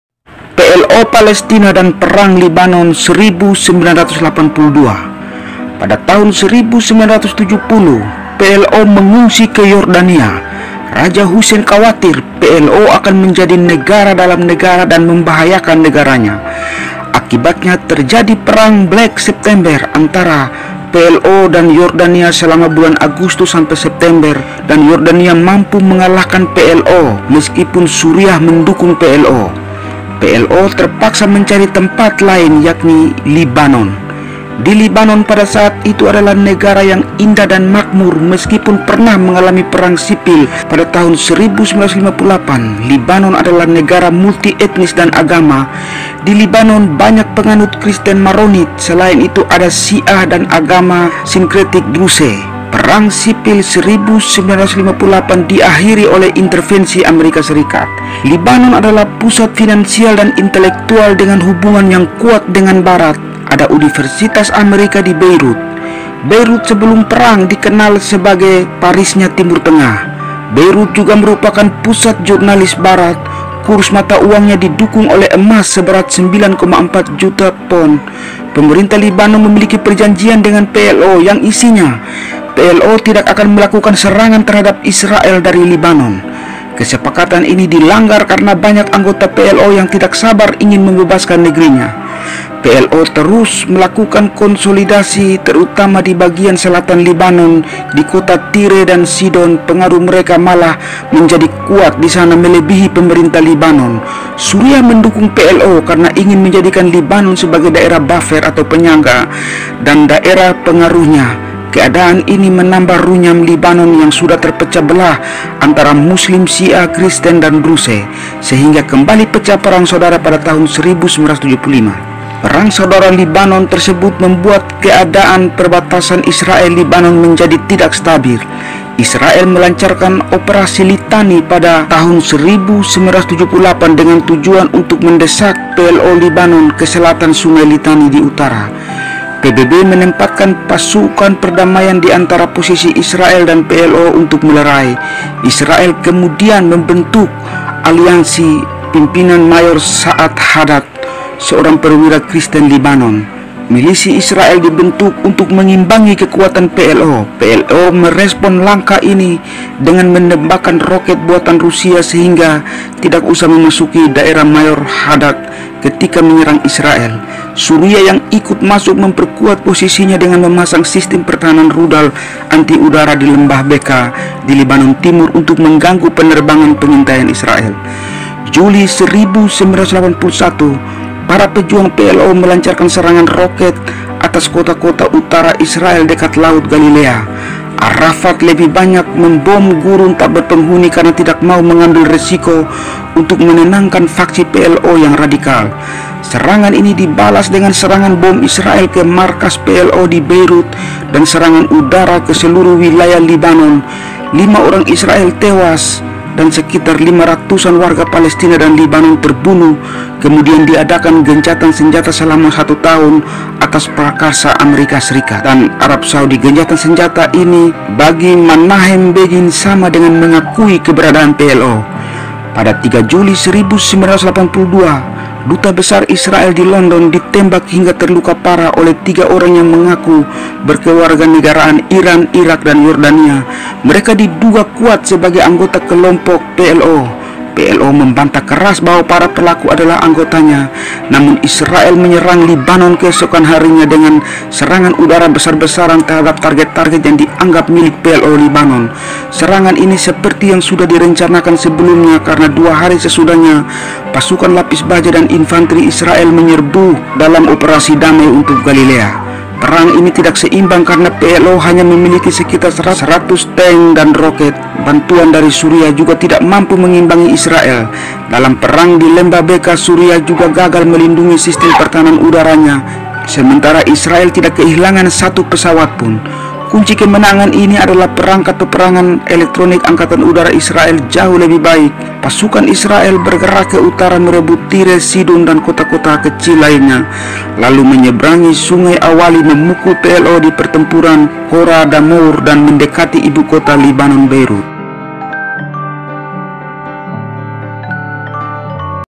PLO Palestina dan perang saudara Lebanon
setelah terusir dari Yordania, PLO Palestina mengungsi ke Lebanon..sepak terjangnya PLO Palestina di Lebanon melahirkan perang saudara Lebanon..

video terkait :
sejarah perang PLO Palestina vs Yordania